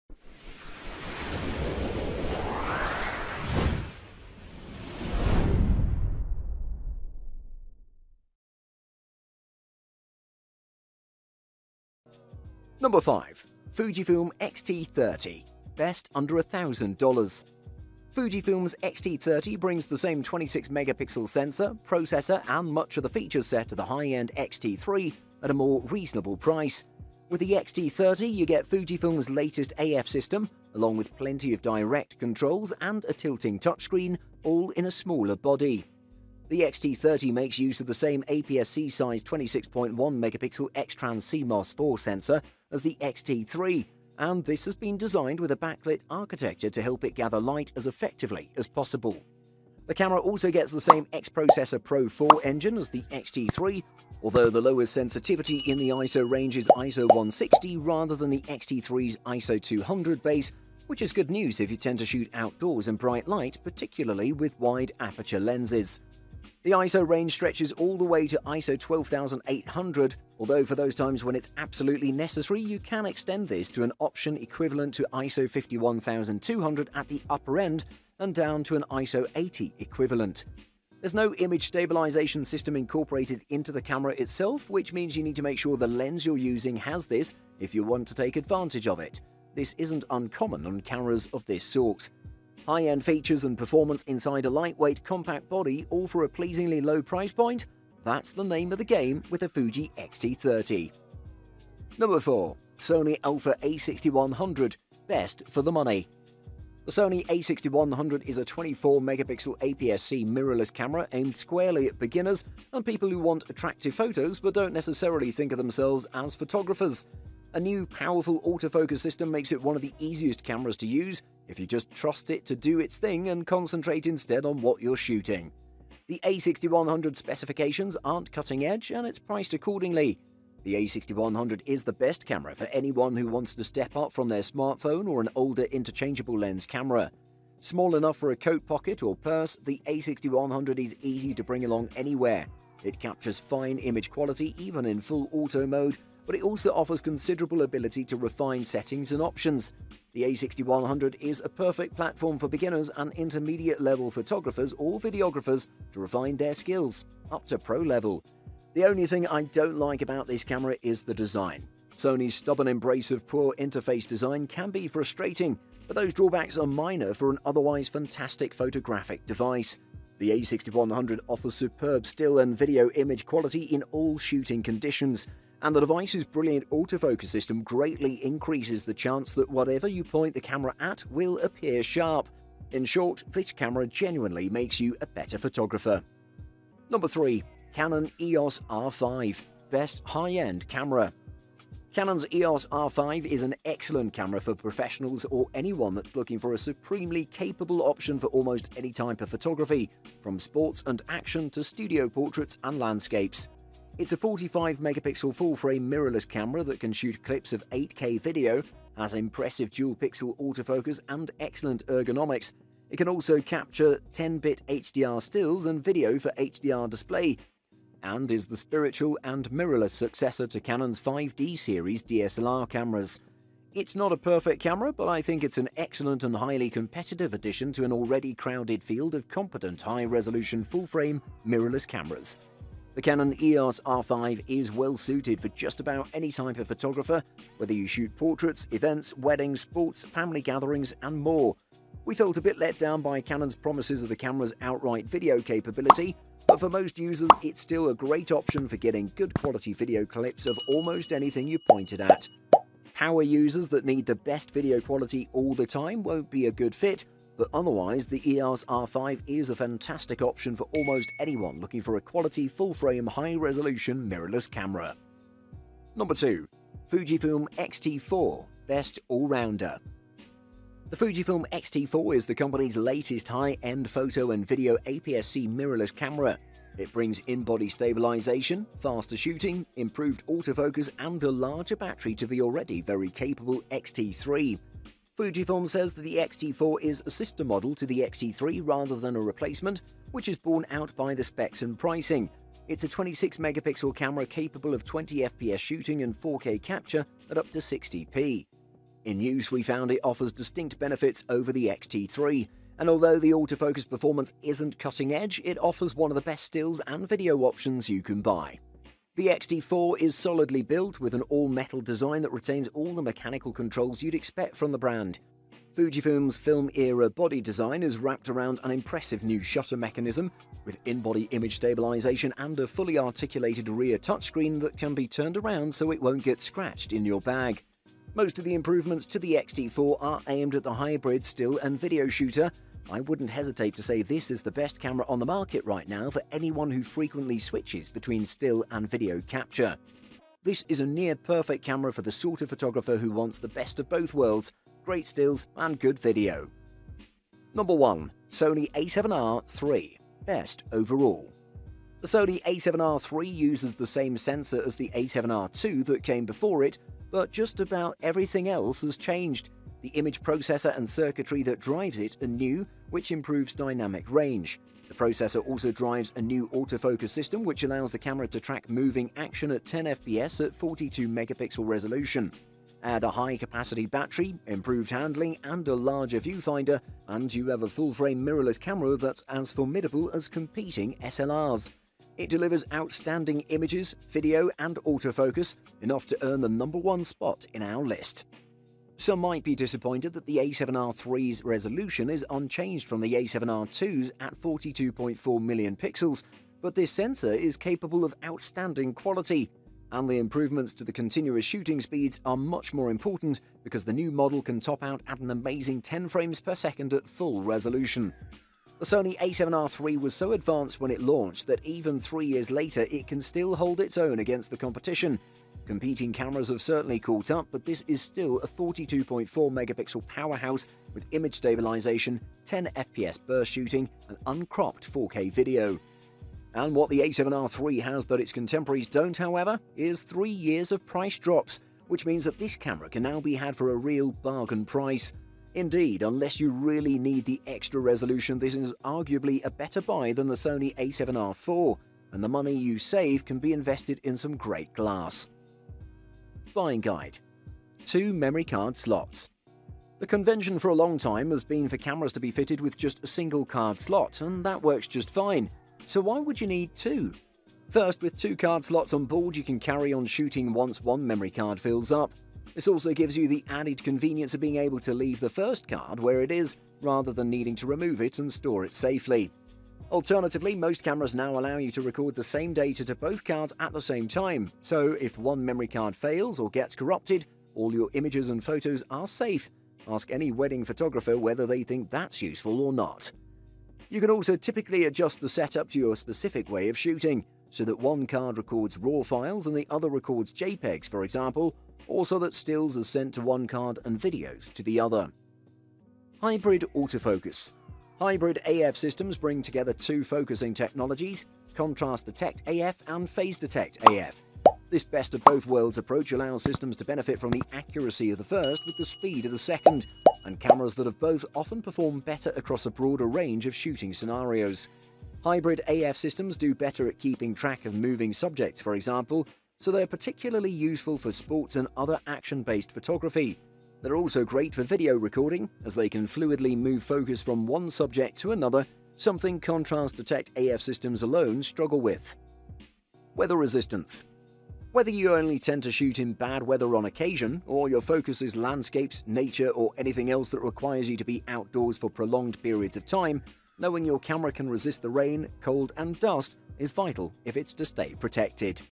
✅ 2021 Review Canon EOS R5 Full Frame Mirrorless | Top 5 Best Mirrorless Cameras 2021
► Links to the Best Mirrorless Cameras 2021 we listed in this video:

► 5 - Fujifilm X-T30
► 4 - Sony Alpha A6100
► 3 - Canon EOS R5
► 2 - Fujifilm X-T4
► 1 - Sony a7R III